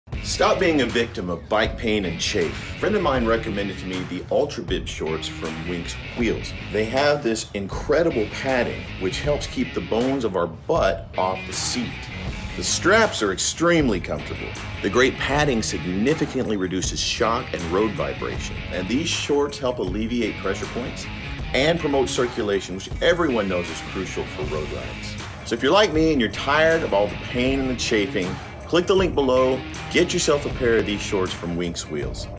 Ready for a ride without limits?
Winx Ultra Cycling Bib Shorts provide the support and flexibility you need to push your boundaries and reach new heights.